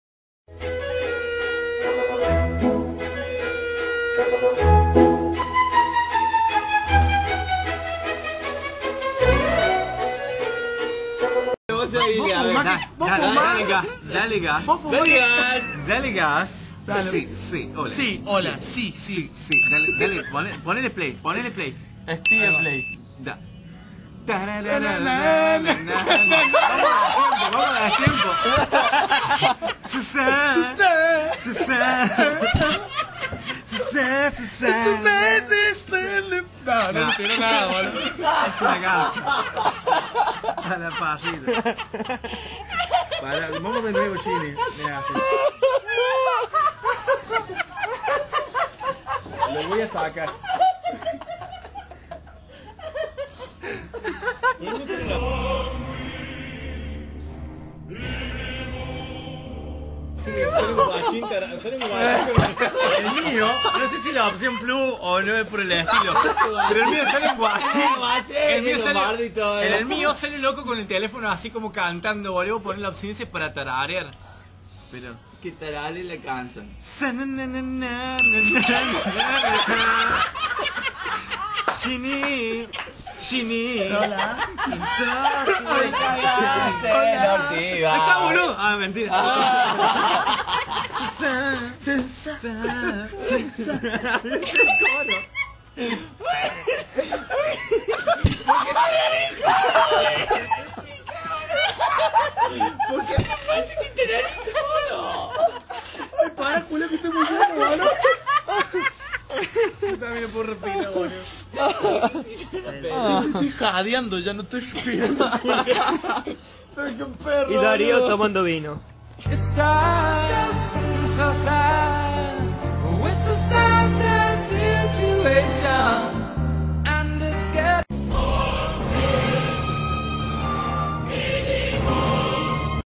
Averiguando la canción con Shini y Mefis
Cuando cenábamos Shinigami se acordó de una canción pero no de su nombre, estos son los intentos para tratar de averiguar su nombre.